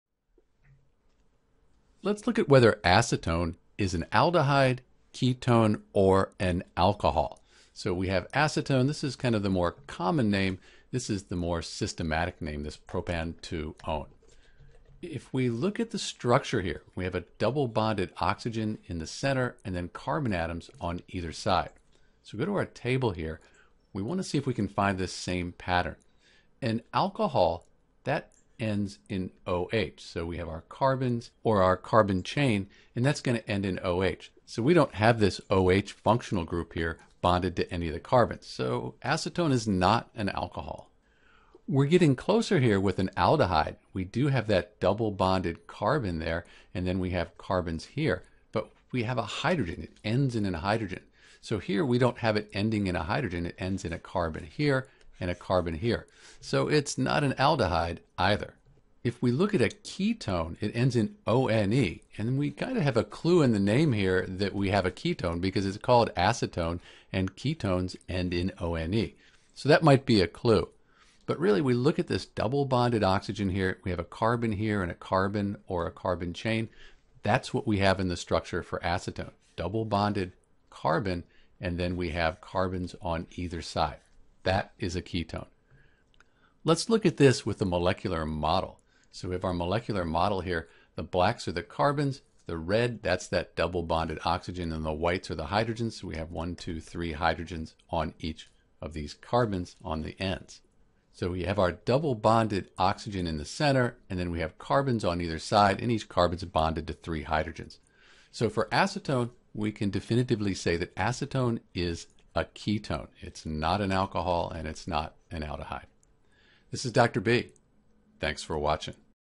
Is Acetone an Alcohol, Ketone, or Aldehyde?
In this video we'll figure out whether Acetone is an alcohol, ketone, or aldehyde using a table listing common functional groups.

Acetone has a Carbon atom double bonded to an Oxygen atom.  On either side of the Carbon atom is another Carbon atom.  This makes Acetone a ketone.  In fact, looking at the name Acetone you can see the it ends in -one, just like the functional group Ketone.

An alcohol would have an -OH functional group (which Acetone does not have).  An aldehyde would have the double bonded Carbon atom and another C on one side and an H atom on the other.  So Acetone is not an aldehyde either.

For a complete tutorial on naming and formula writing for compounds, like n-Propanol (1-Butanol) and more, visit: